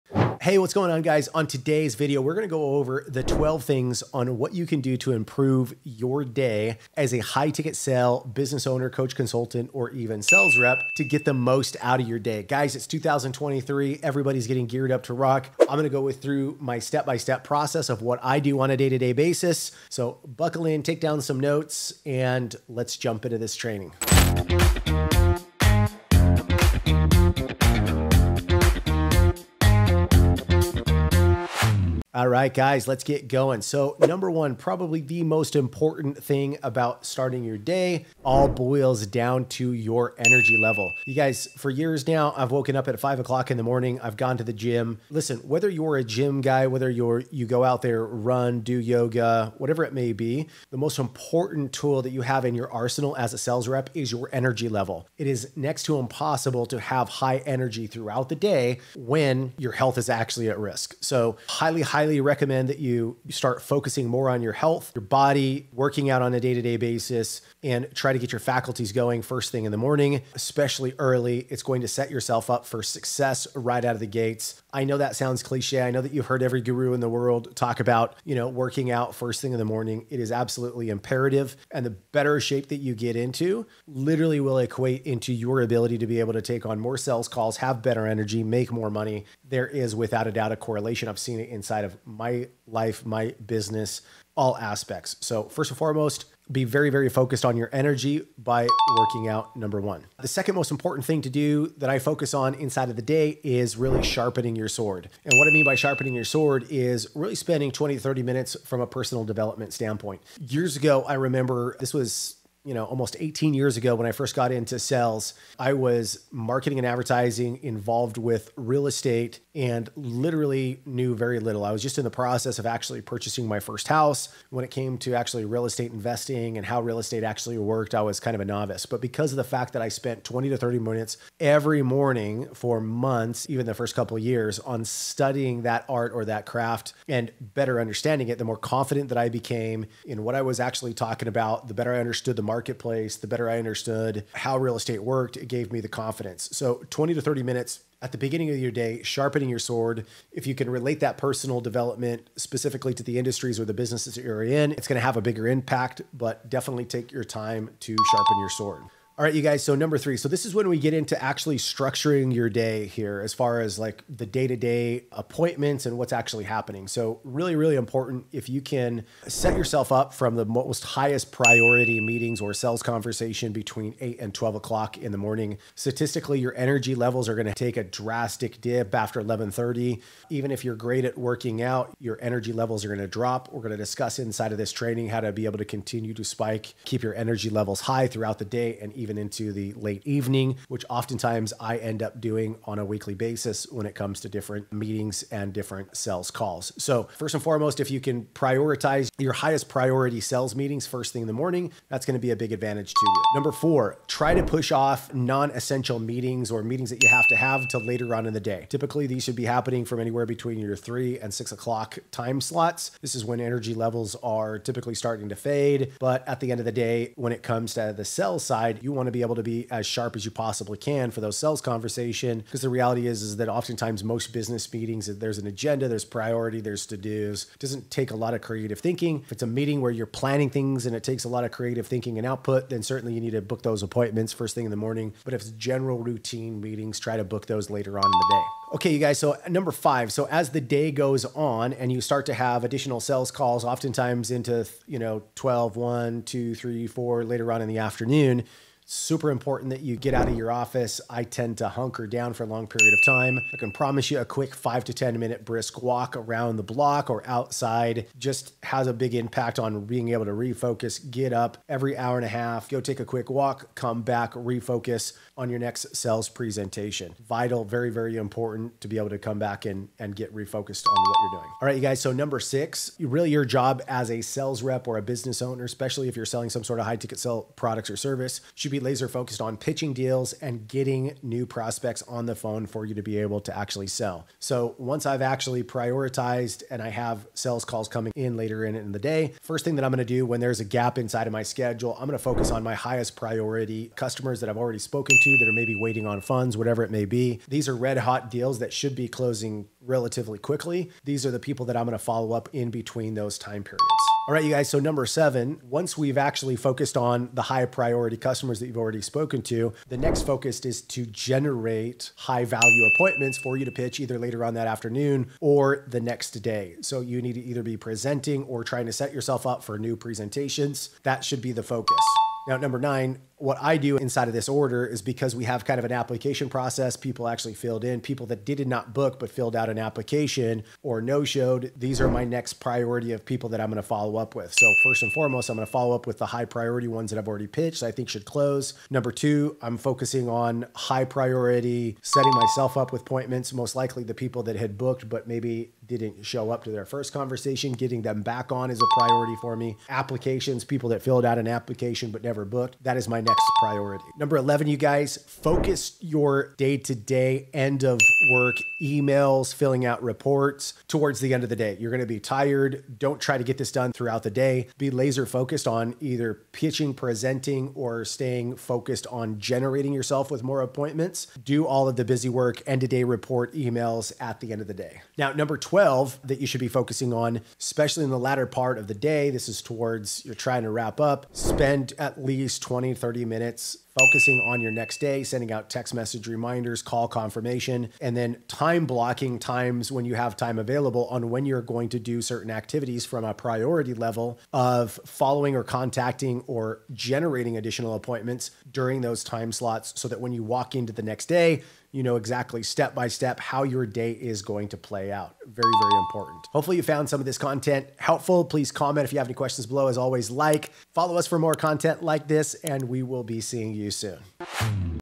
12 Things To Manage Your Day as a High Ticket Sales Rep
Here are 12 things you can do to get the most out of your day when closing high ticket deals:

1. Boost your energy - Sales is energy and if your energy is low your commissions will be low.

2. Sharpen your sword and spend at least 30 min in personal development if you can focus it on your expertise, it will pay off.

3. Focus on your top priority closes 8am to 12 your energy your focus will be best!

4. Try to push meetings and non-closing to the end of the day.

5. Make sure in between high priority calls be sure to get up and refocus your energy quick walk can go a long way.

6. In between high priorities calls be sure to focus on deals that are closes to close.

7. Your next priority is to focus on generating high value appointment.

8. No shows

9. Follow ups applications that didn't book.

10. Follow ups on leads that never booked.

11. At the end of the day focus on end of day reports and any emails that have to go out when energy is lowest.

12. Confirm appointments for the next day.